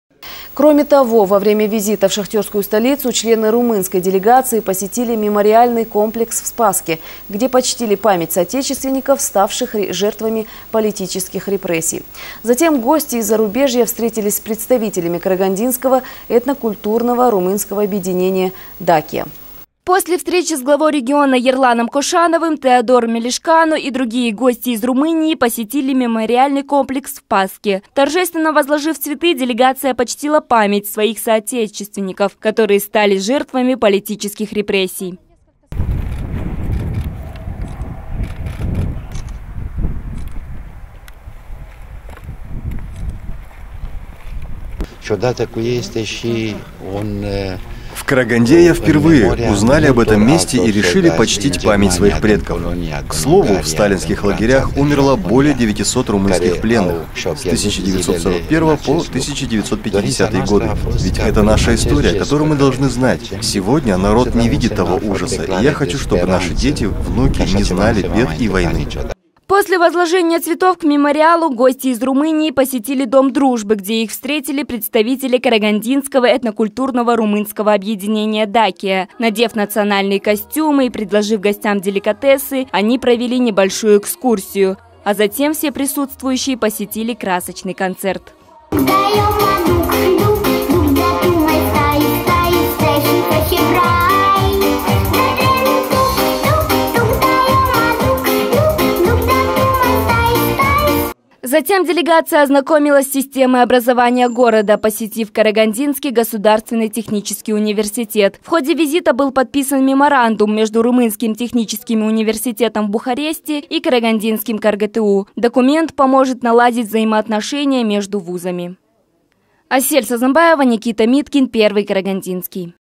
Караганду посетила делегация из Румынии.
Кроме того во время визита в шахтерскую столицу члены румынской делегации посетили мемориальный комплекс  в Спасске, где почтили память соотечественников, ставших жертвами политических репрессий.  Затем гости из зарубежья встретились с представителями карагандинского этнокультурного румынское объединение «Дакия»
После встречи с главой региона Ерланом Кошановым, Теодор Мелешкану и другими гостями из Румынии посетили мемориальный комплекс в Спасске. Торжественно возложив цветы, делегация почтила память своих соотечественников, которые стали жертвами политических репрессий.
После возложения цветов к мемориалу гости из Румынии посетили дом дружбы, где их встретили представители карагандинского этнокультурного румынское объединение «Дакия». Надев национальные костюмы и угостив гостей деликатесами, они провели небольшую экскурсию. А затем все присутствующие посетили красочный концерт.
А также гости из Румынии ознакомились с системой образования города, посетив Карагандинский государственный технический университет. В ходе визита был подписан меморандум  между Румынским техническим университетом в Бухаресте и карагандинским КарГТУ. Документ поможет наладить взаимоотношения между вузами.